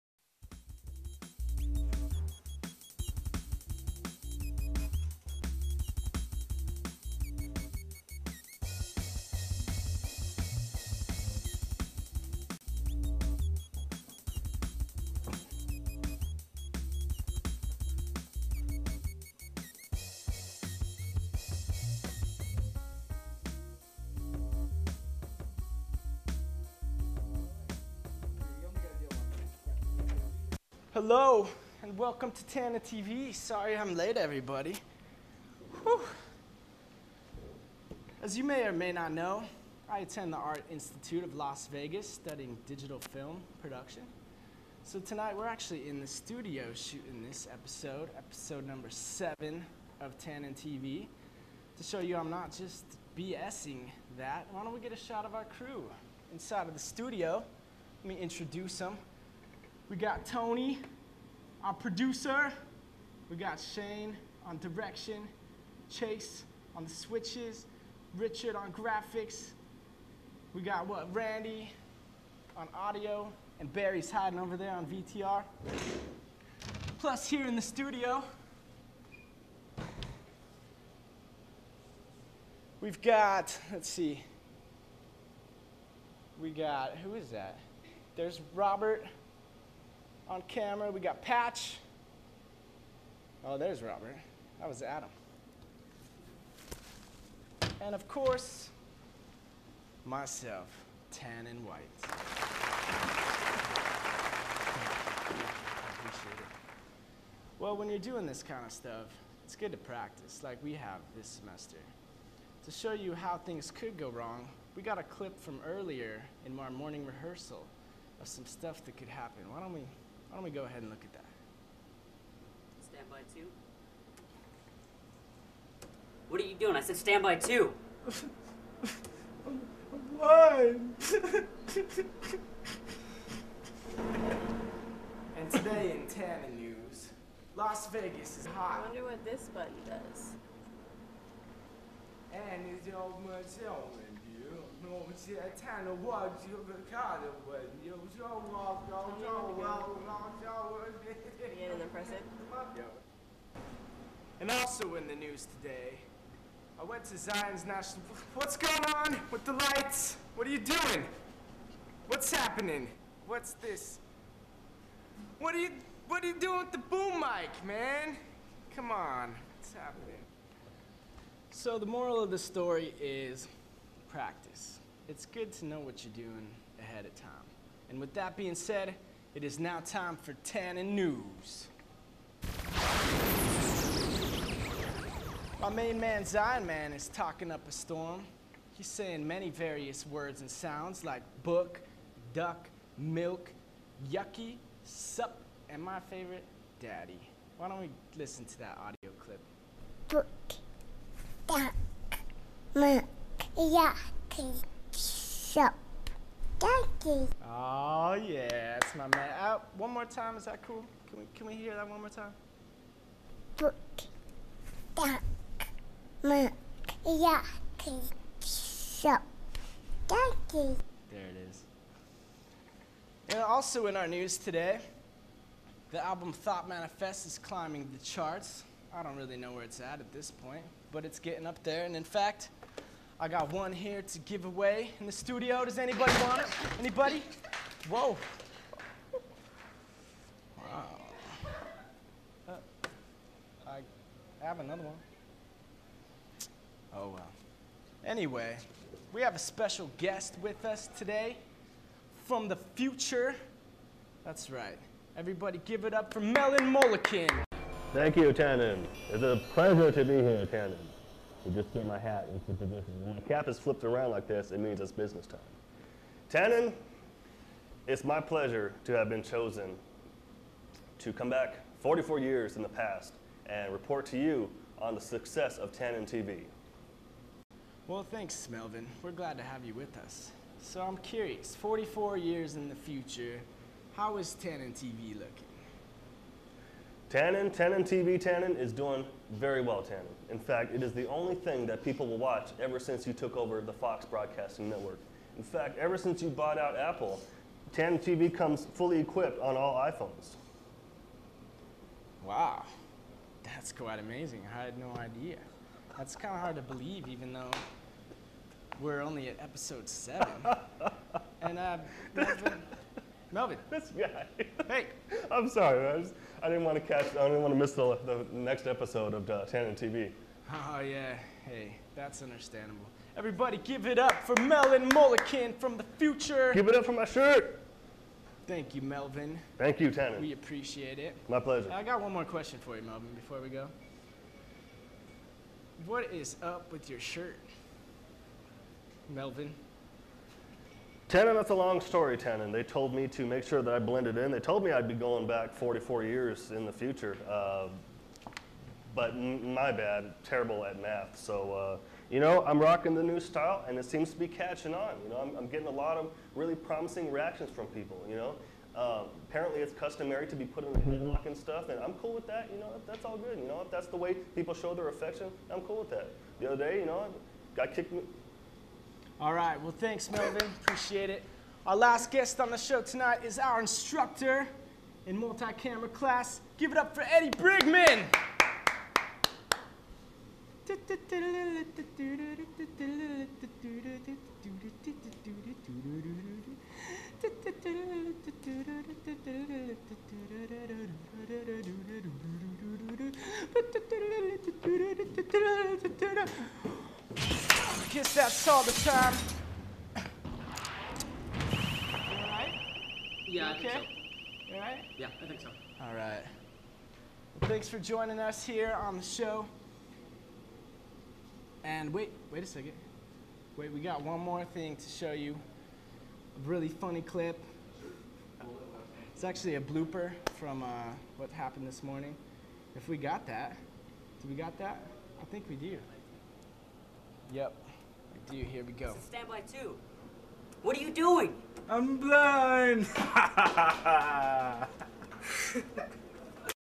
TananTV - Episode #7 Live from The Art Institute of Las Vegas
For my Mulit-Cam class final we shot this episode of Tanan TV live in the video studio at AILV.  Props to my crew: Shane, Tony and Patch, and the rest for a job well done.